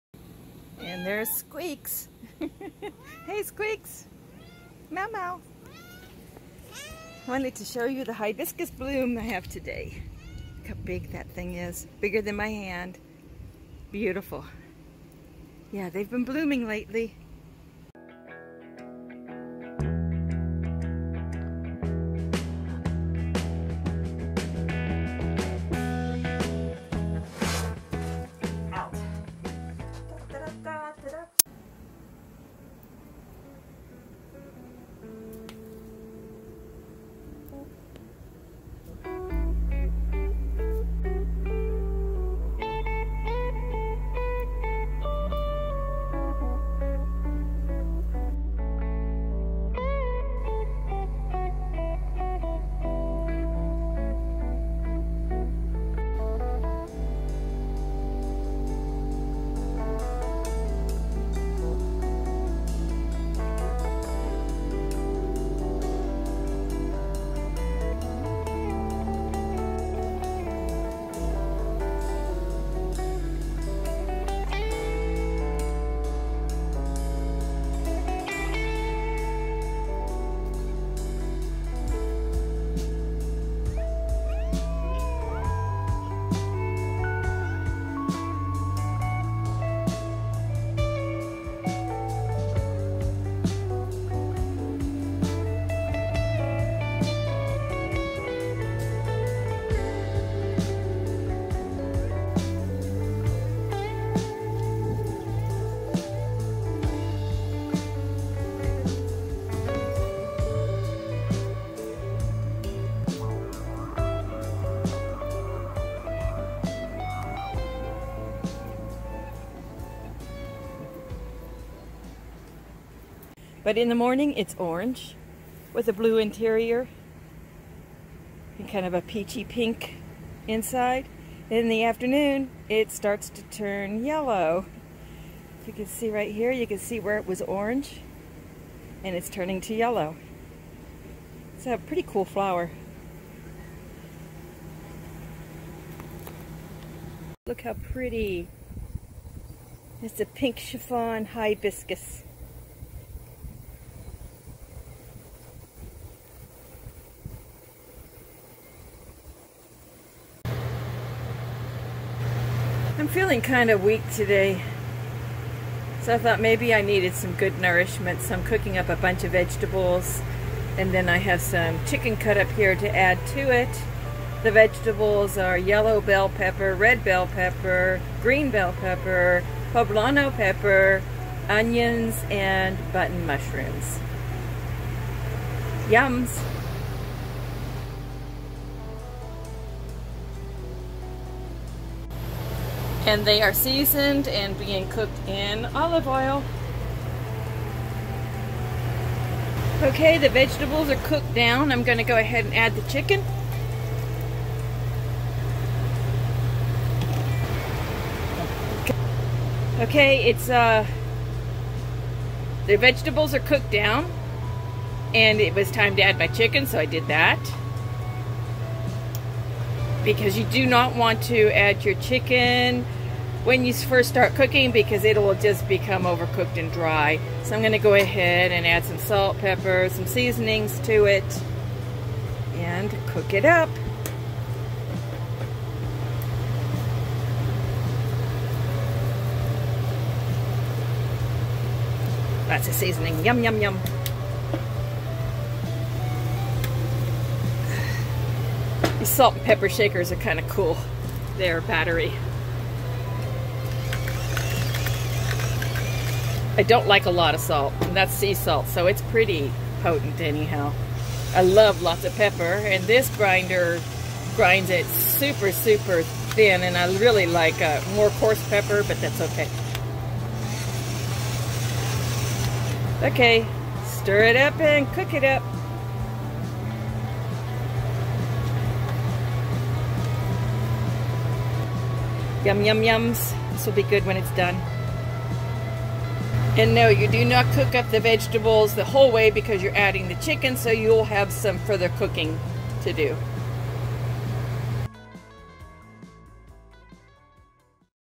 FULLTIME RV LIFE Beautiful flowers and VEGGIE CHICKEN STIR FRY
Just a video display of my beautiful flowers, then onto cooking some yummy stir fry chicken and veggies!! If you want to suggest spices for my stir fry, I am all ears!